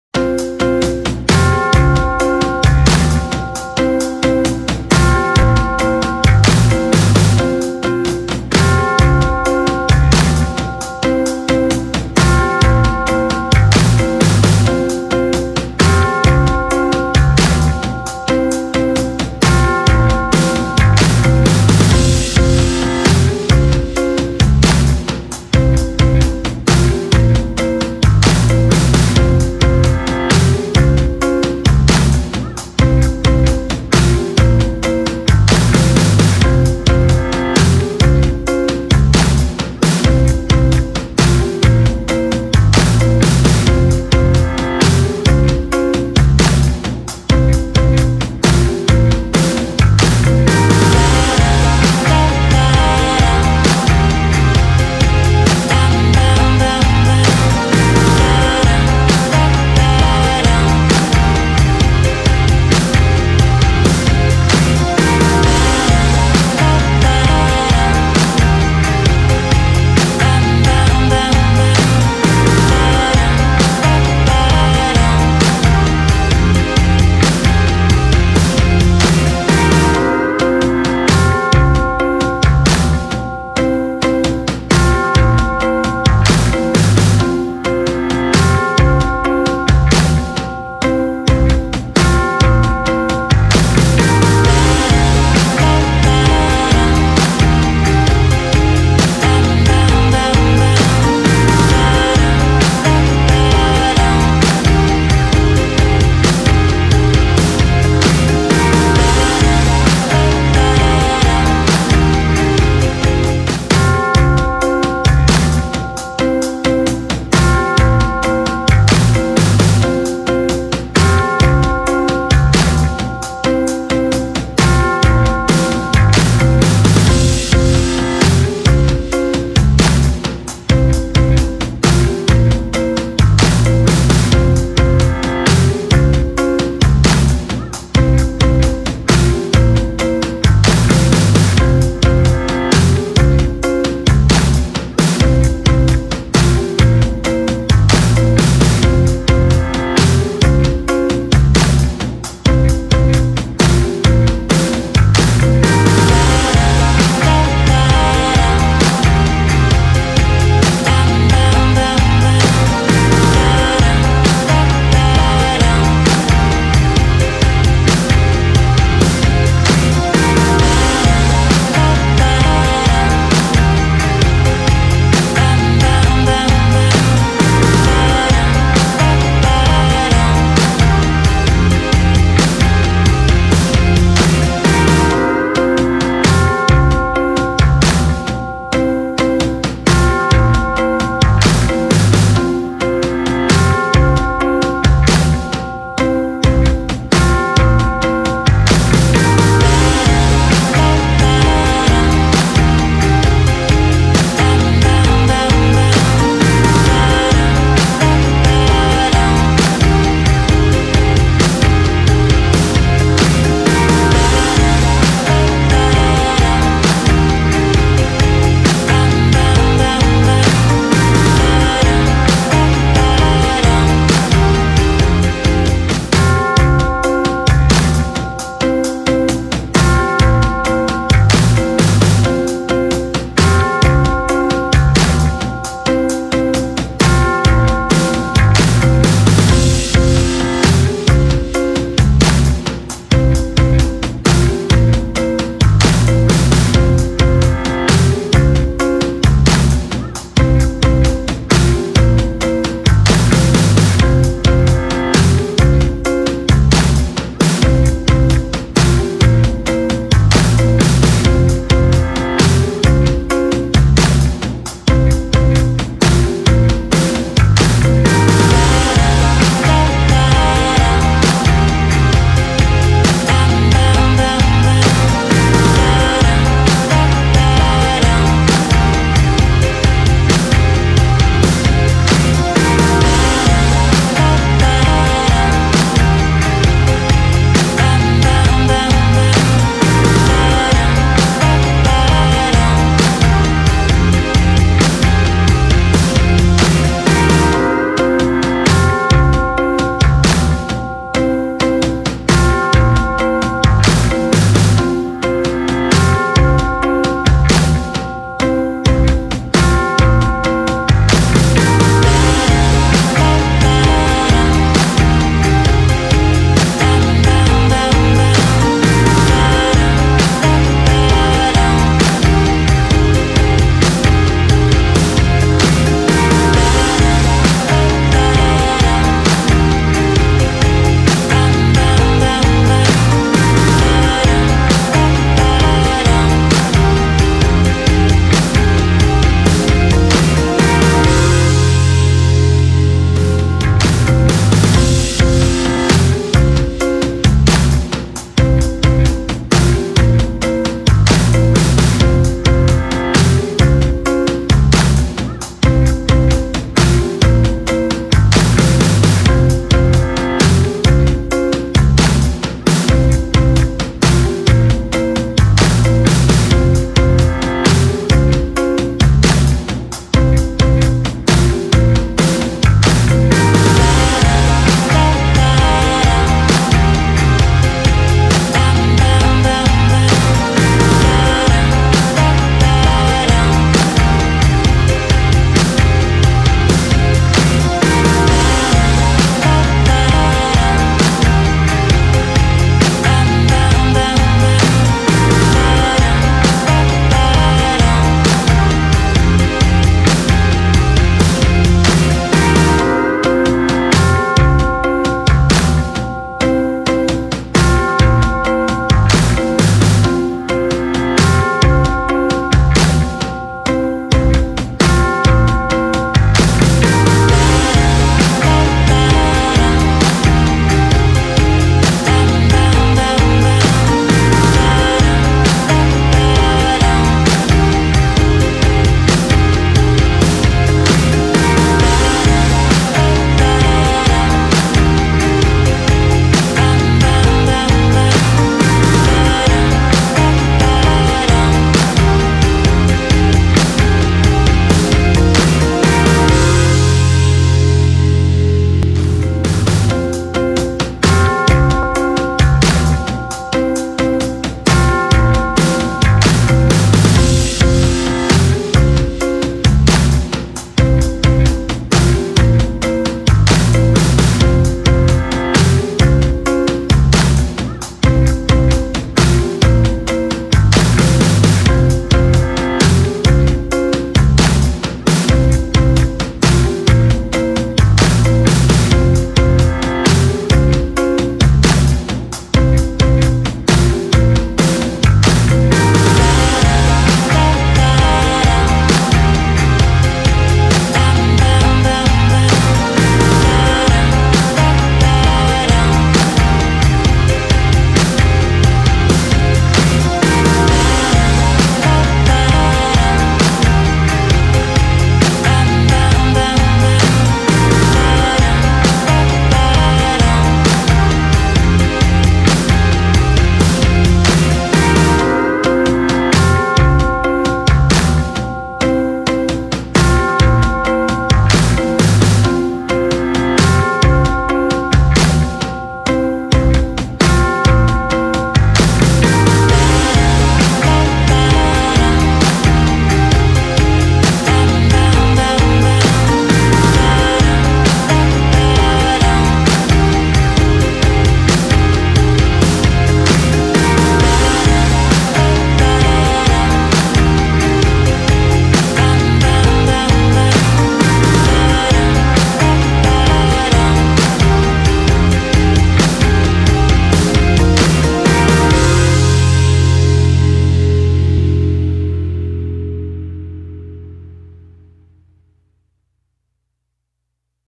2010s General Trivia Quiz #2 / Anything Goes for the Decade! / 25 Qs
2010s General Trivia Quiz #2 / Anything Goes for the Decade! / 25 Qs

Today's episode is anything to do with the decade of the 2010s.
The 10s aren't that far behind us, but do you remember anything?